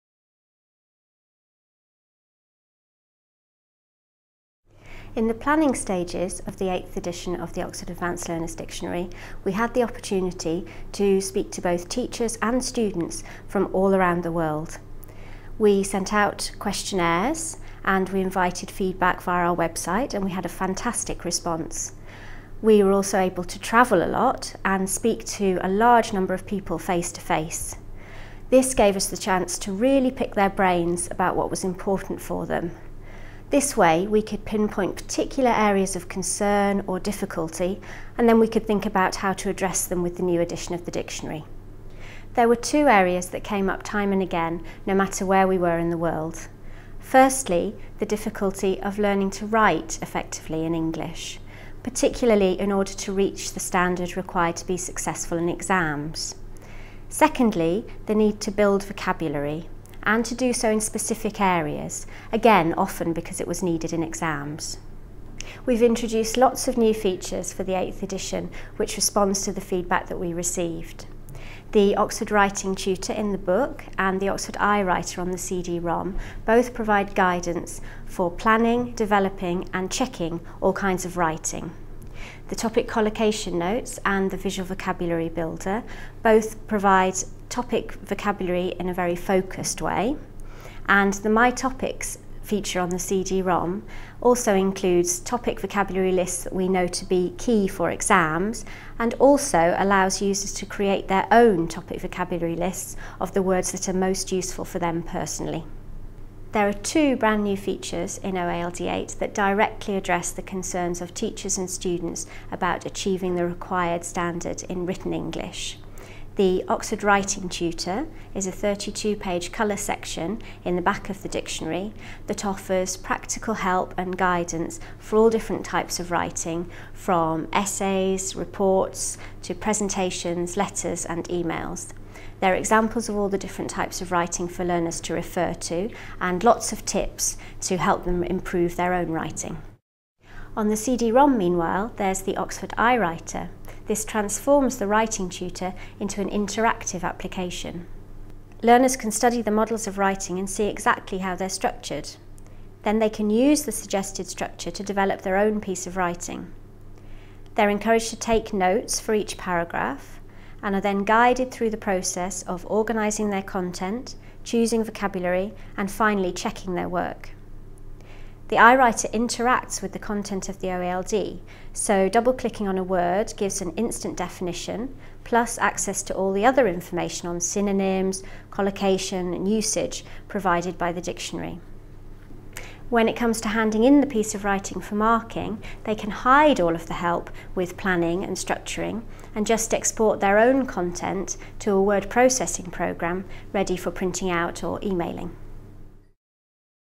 What's new for OALD 8th Edition?
Joanna Turnbull, ELT Dictionaries Managing Editor, explains what's new in the Oxford Advanced Learner's Dictionary 8th Edition